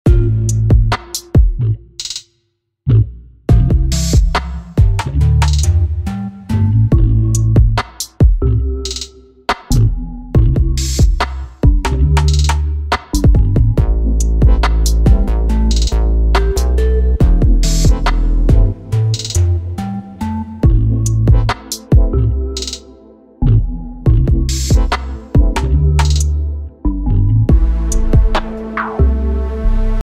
AI Made Art & Music
Image made with JuggernautXL, Upscaled with gigapixel and music made with musicmaker AI